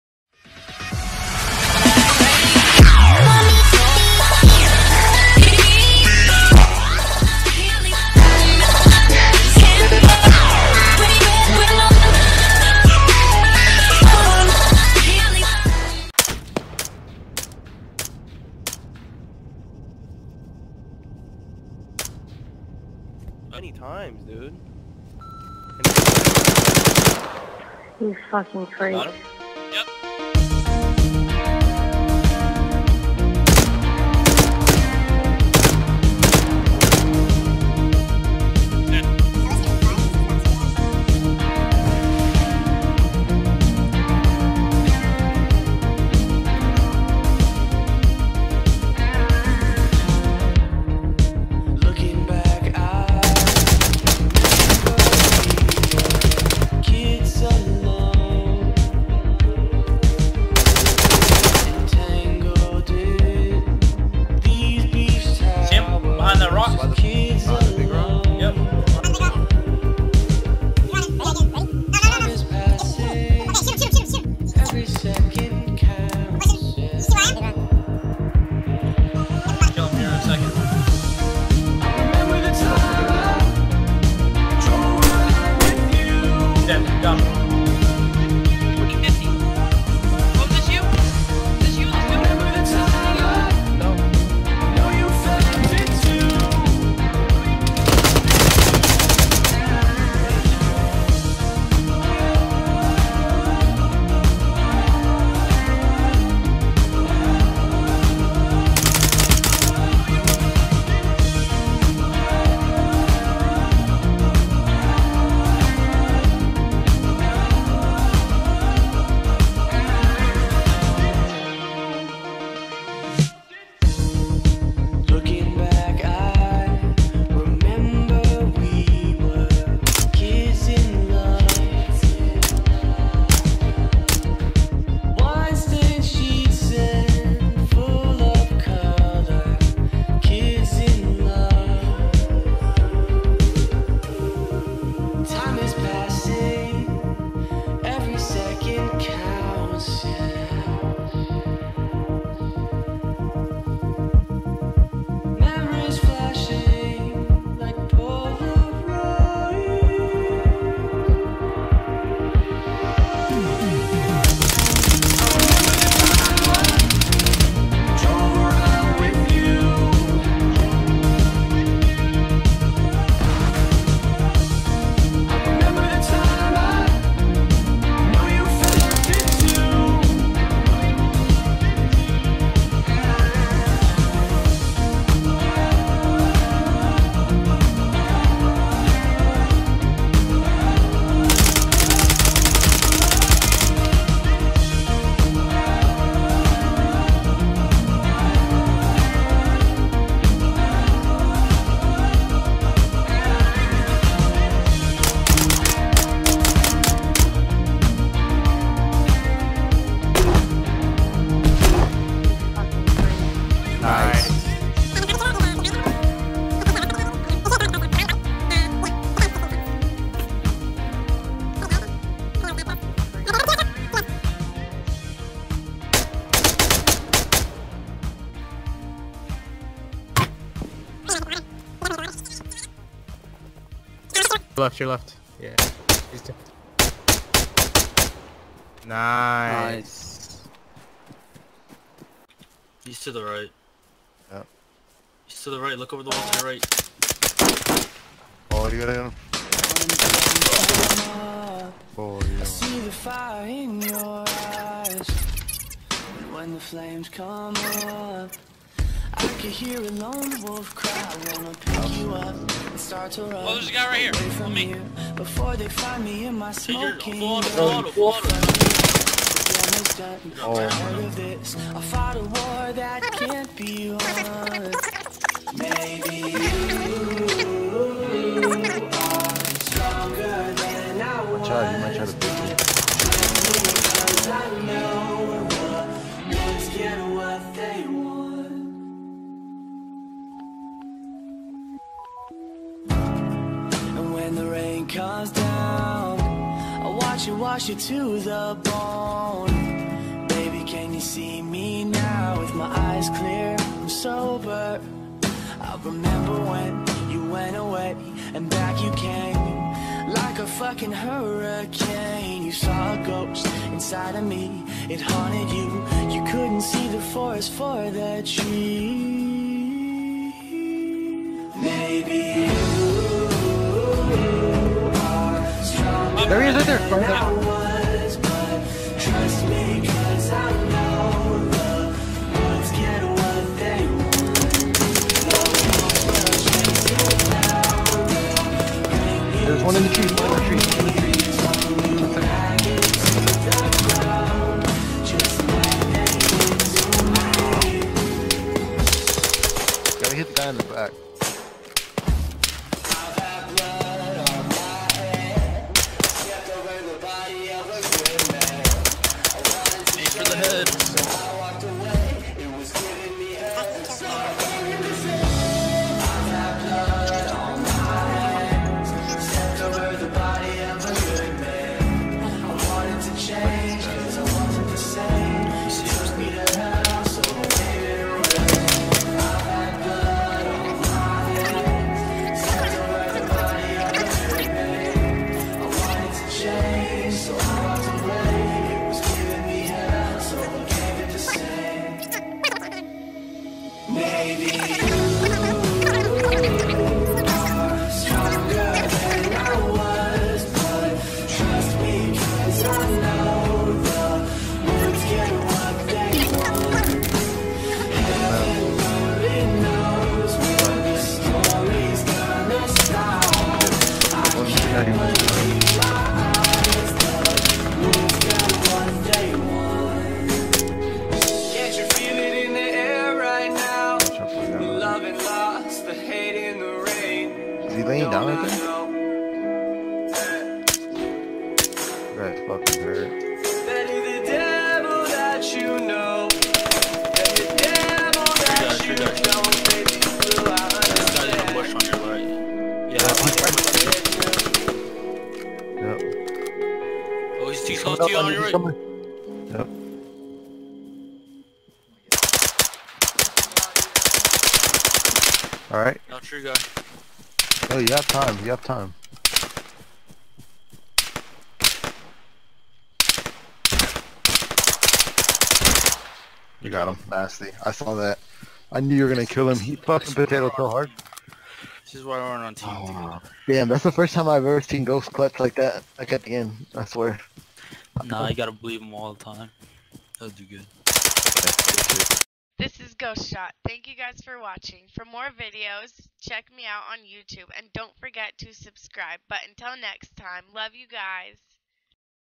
Ghostshot battle royale montage
few good moments since i started playing with Brad and Astro